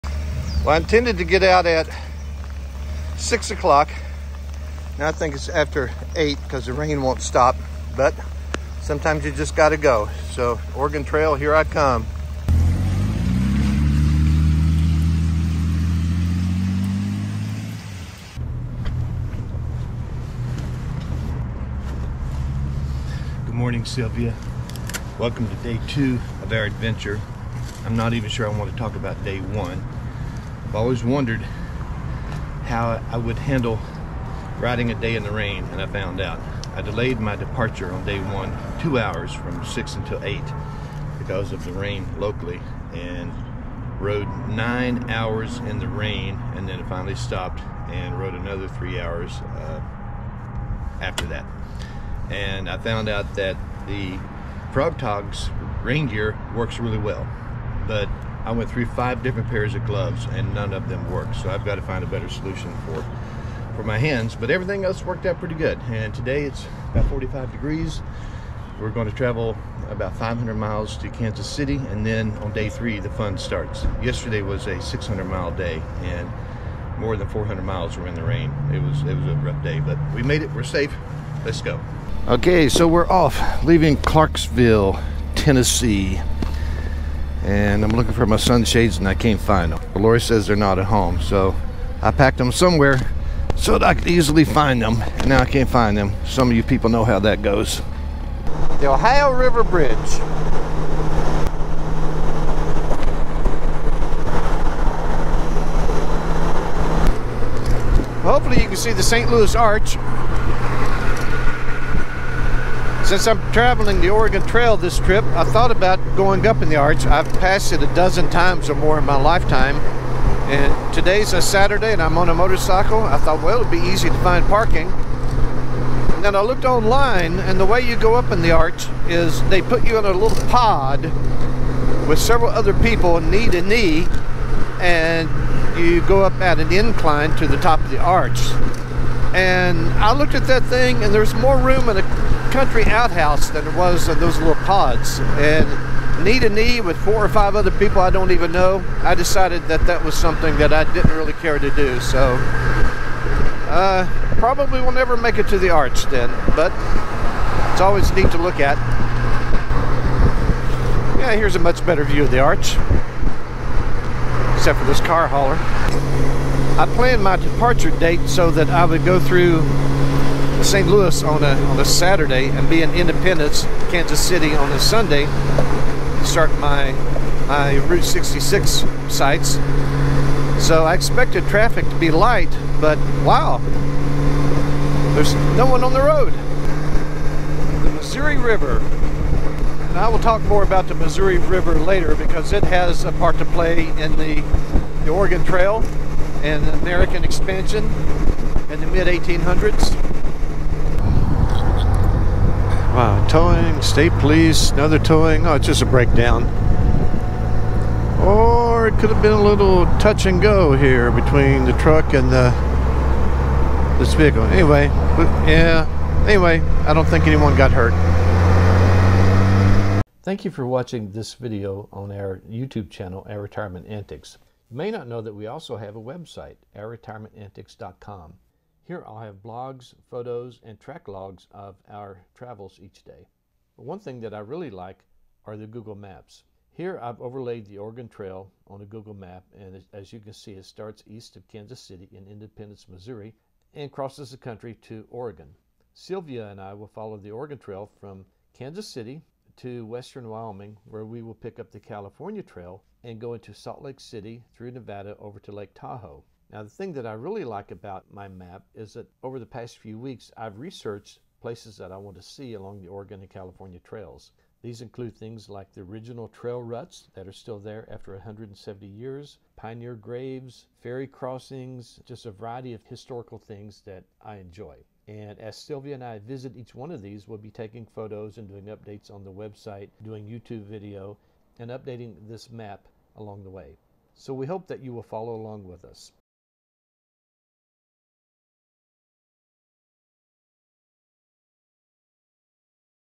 Silvia and I Head Out for the Oregon Trail in the Rain
The Oregon/California/Mormon/Pony Express Trails fascinate me. What drove those people to leave their homes and extended family to head into the unknown? Roughly 500,000 people travelled west in a 30 year period by wagon.

10% of them died. There are roughly 15 graves for every mile of the trail. If you enjoy history or motorcycle videos, please join the journey with us.

I have very little video for day 1 as it rained 9 out of the 12 hours we were on the road. Most of this video is for day 2 getting to Independence Missouri which is the official start of the trail. The real fun starts in the day 3 video where we start visiting trail sites.